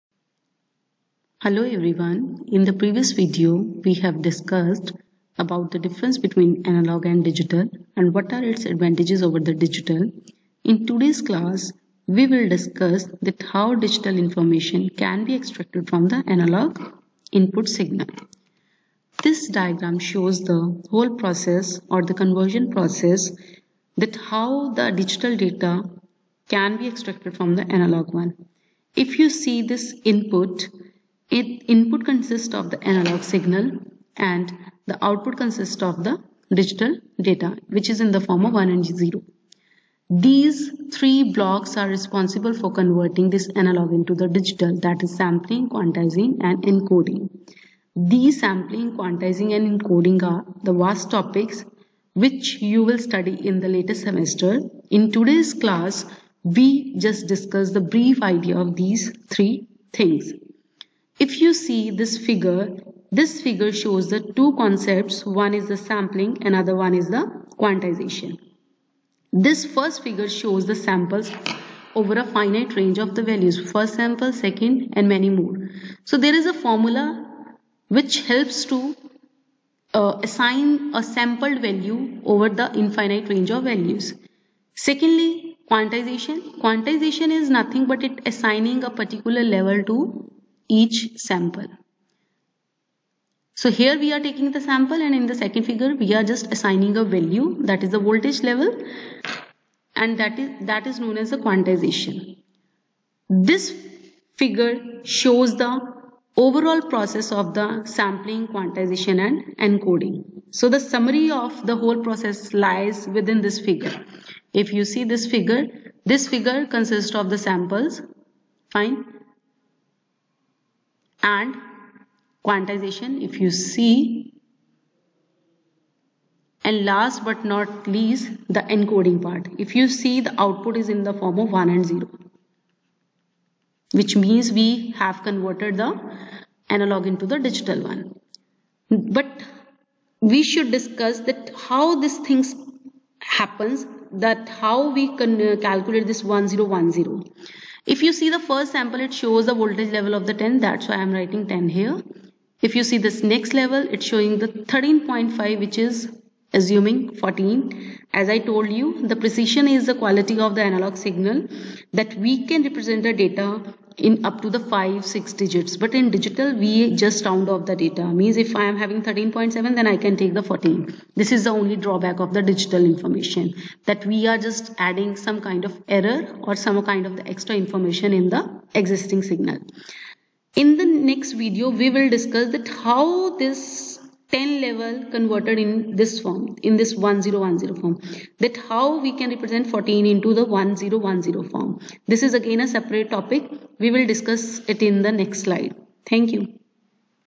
Analog to Digital Conversion by Ms. Rubina Dutta
This video gives knowledge about the  need for conversion of analog and digital signal from one form to another, which marks the prerequisites for introducing digital electronics.